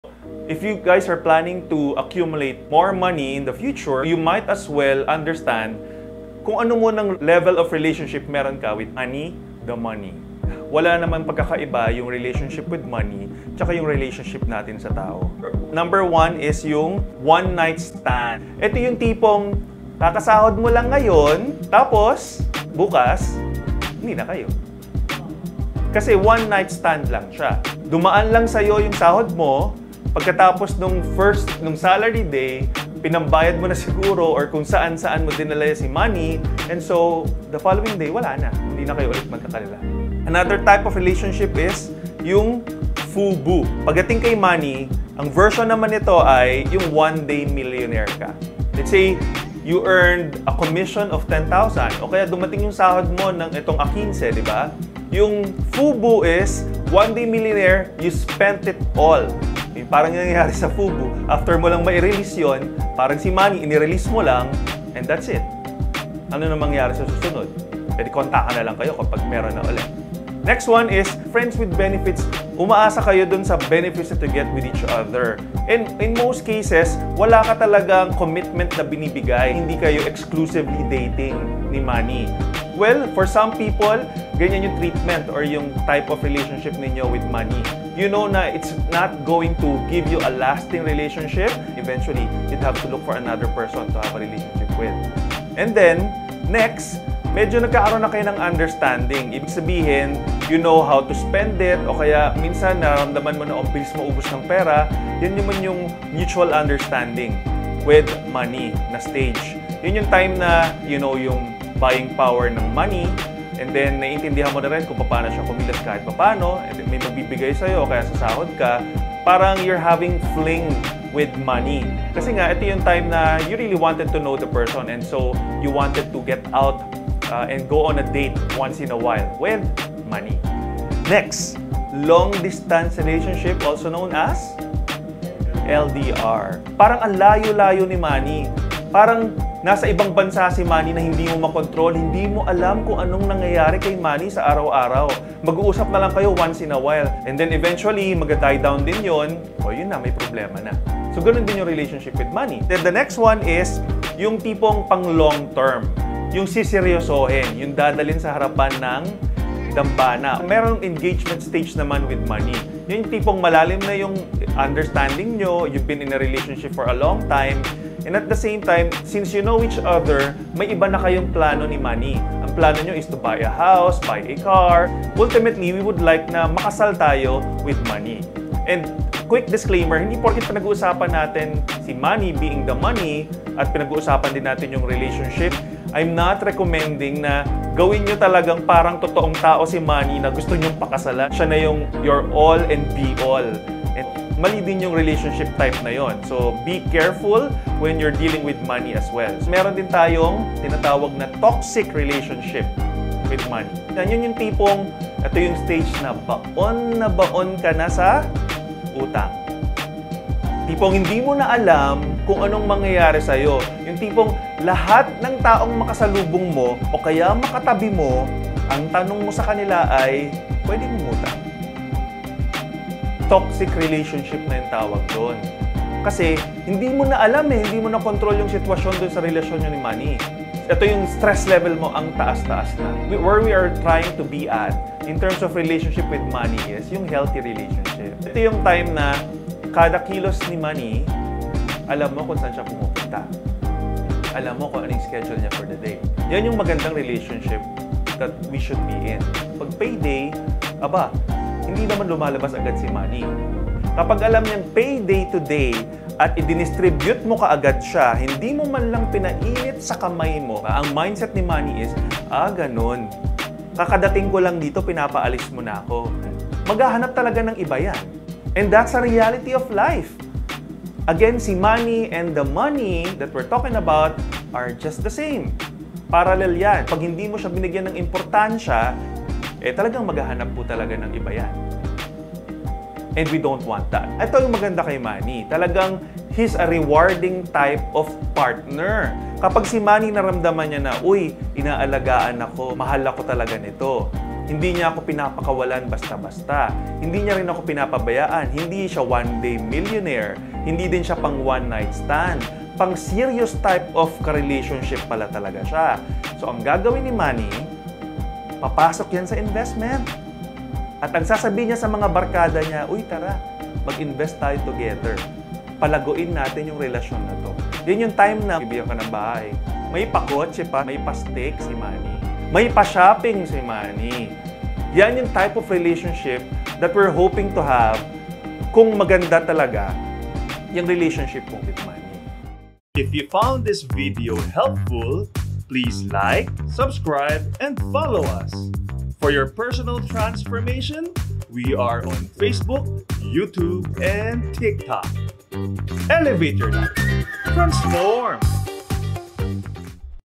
How to Manage Money better? Financial Management for beginners
How to Manage Money Better? Rules of Money are similar to any relationship. Treat them like your spouse and they will return the favor. This is how one must manage their money to arrive at a point where you become abundant. This is Financial management even for begginers

Relationship Types:
-One night stand
-FUBU
-Friends with
-M.U
-LDR
-Engagement
-TOXIC
-Healthy Relationship